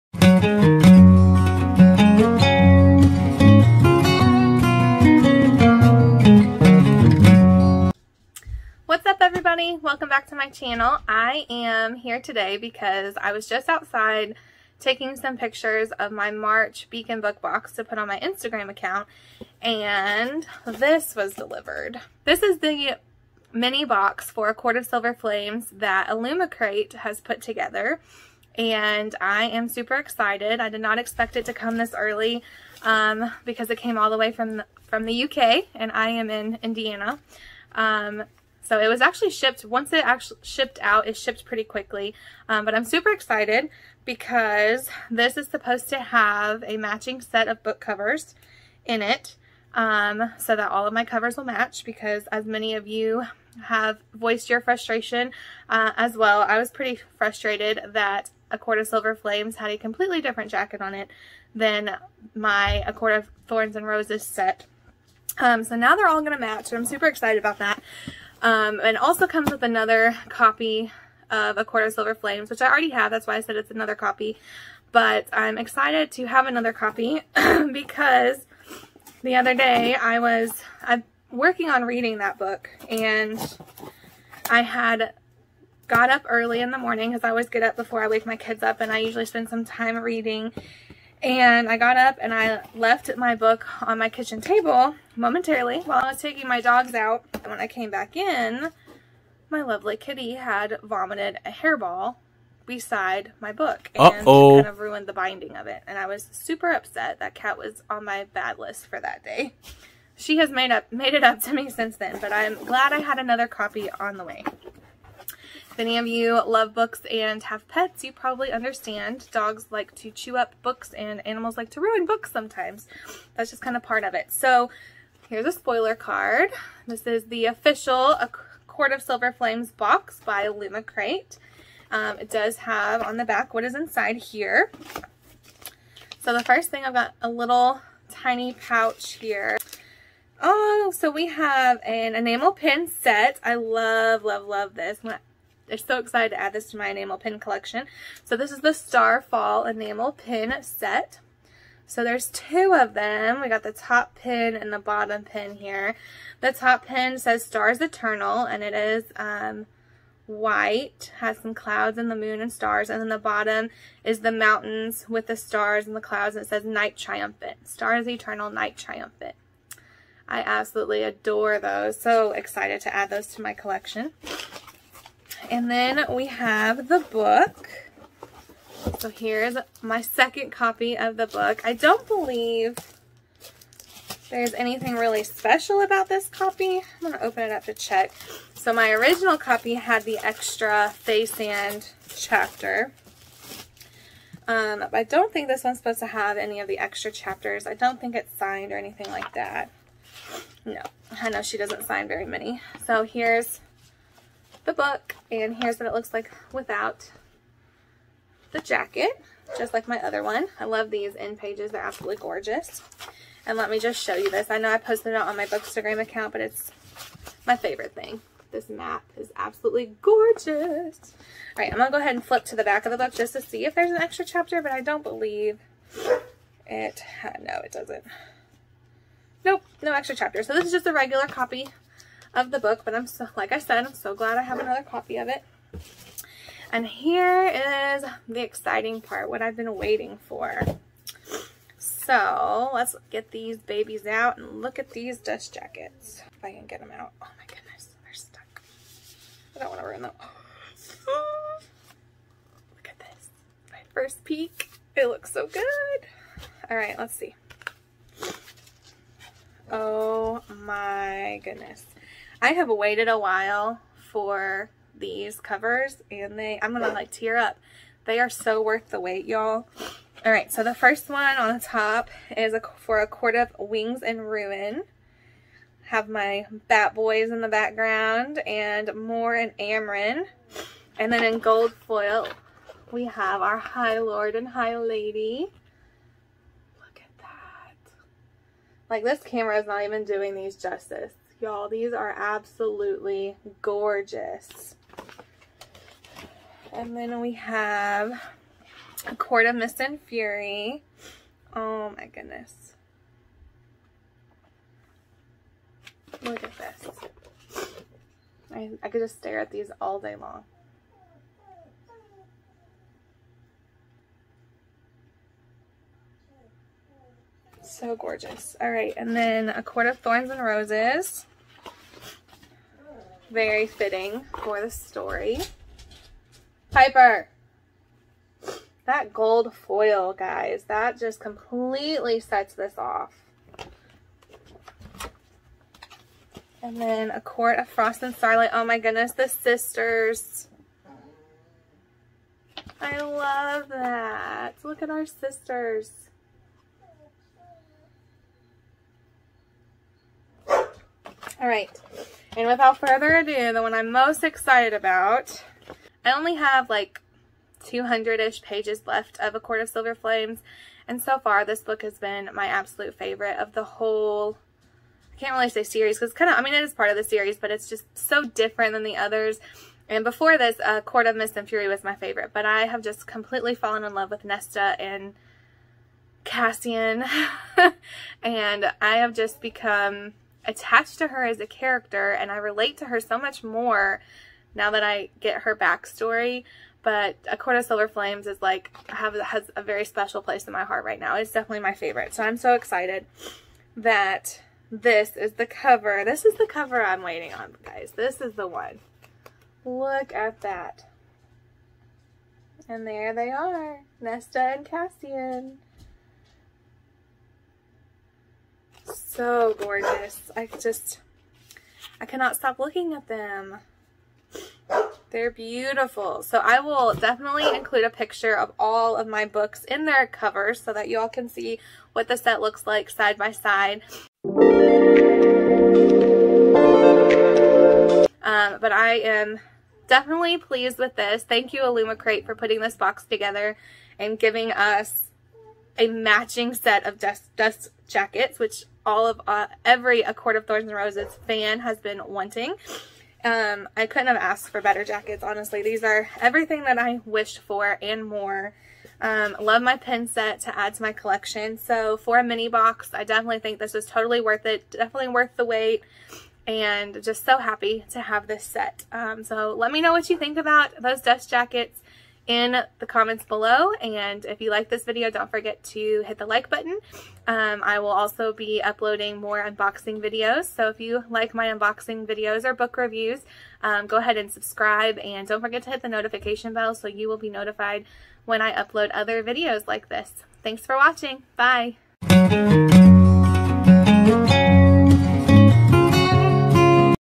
UNBOXING: A COURT OF SILVER FLAMES BOX by ILLUMICRATE
Hey, booklies!

I am so excited to share this UNBOXING with you. I'm sure many of you ACOTAR fans out there were just as frustrated as I was when the dust jacket for A COURT OF SILVER FLAMES by Sarah J. Maas didn't match the dust jackets for the original books. However, thanks to ILLUMICRATE our frustration has been solved!!!

This UNBOXING was so much fun as I'm sure you will tell from my reaction. The dust jacket set in this box is absolutely stunning. I couldn't be happier with how they turned out and my book set is absolutely gorgeous with their new jackets. I look forward to reading your comments!

Happy reading!